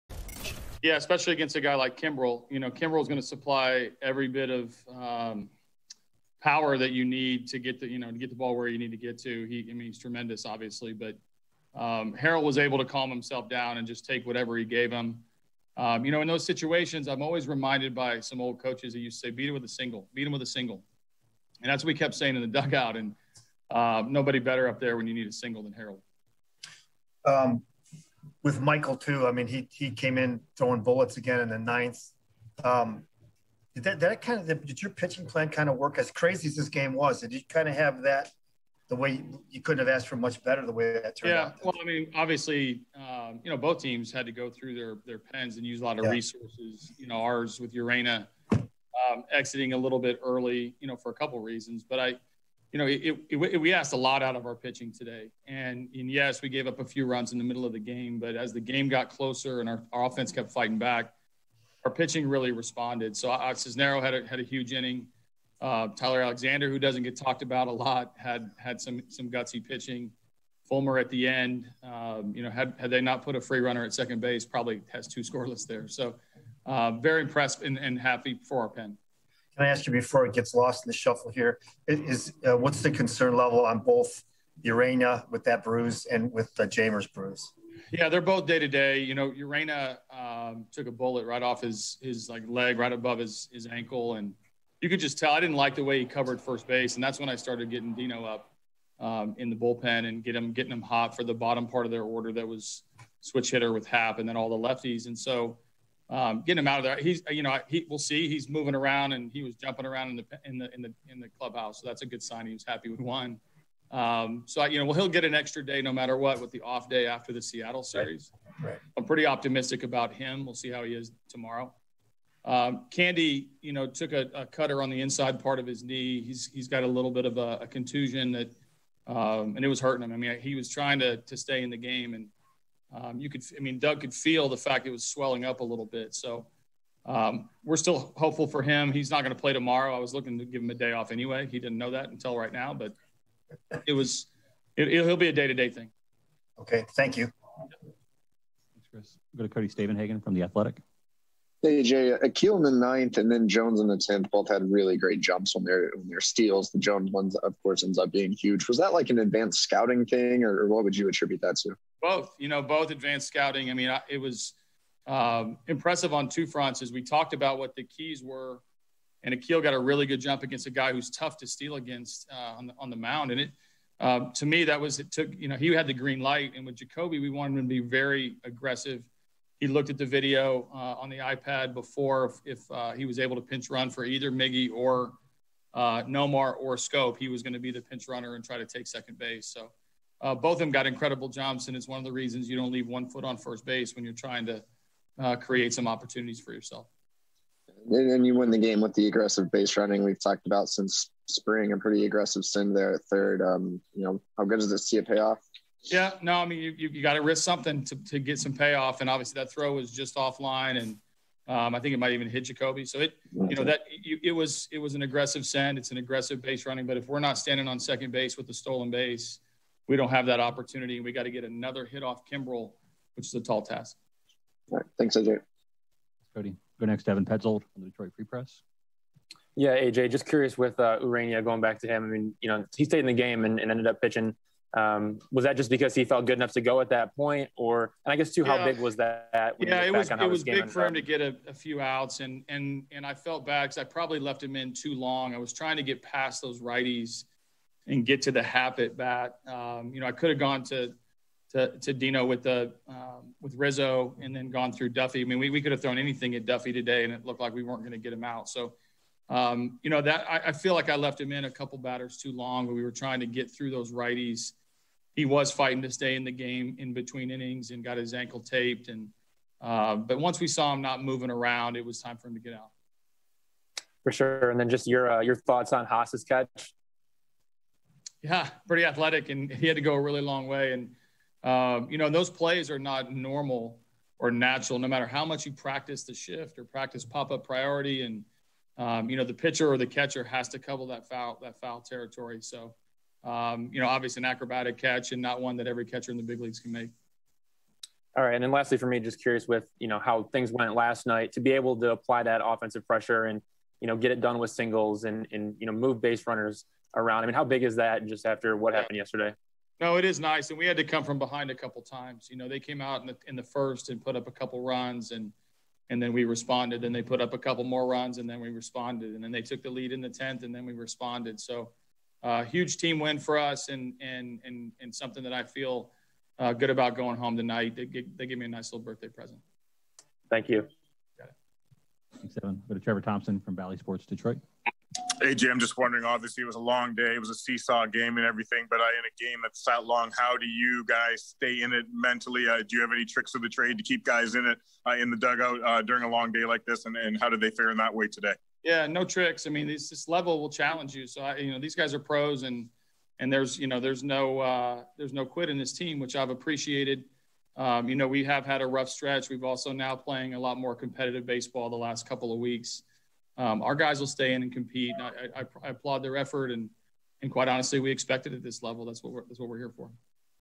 Tigers LIVE Postgame 5.15.21 | A.J. Hinch
Let's get the thoughts of Tigers skipper A.J. Hinch after a back-and-forth, gutsy, and hard earned extra innings win over the Cubs.